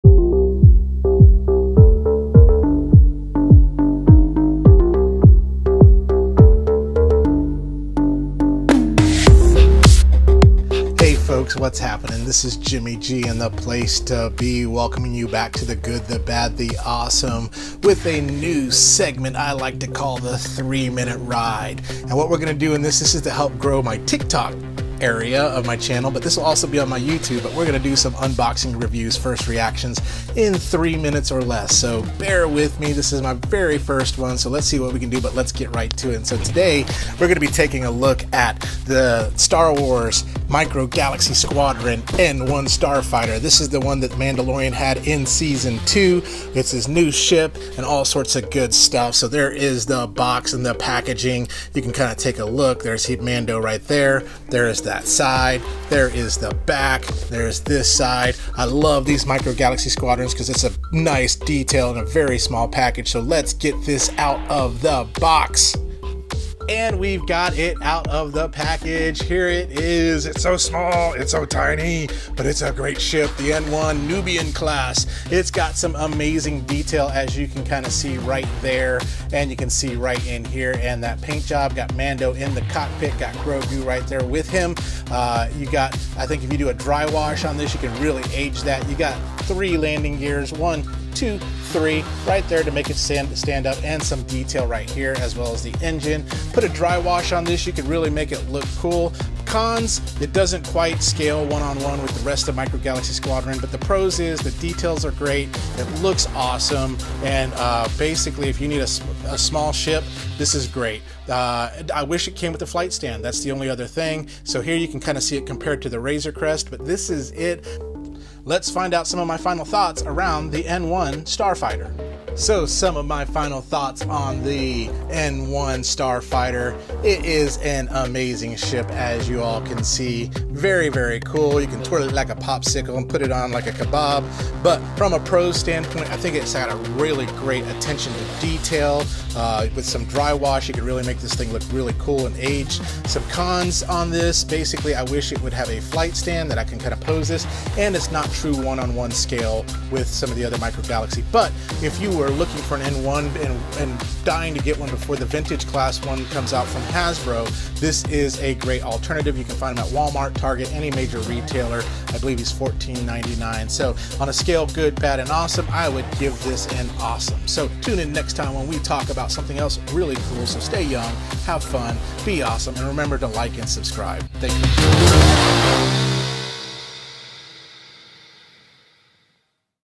Star Wars N-1 Starfighter Mandalorian by Micro Galaxy SquadronReview
Here is a new segment called the 3 Minute Ride.  This is mainly to grow my TIK TOK channel but you all shouldn't miss out as well.  Today we look at the Star Wars Micro Galaxy Squadron N1 Starfighter from THE MANDALORIAN.  Is this ship Awesome?  Let's find out!

Thanks again for all the support.  Click on the link to find out more: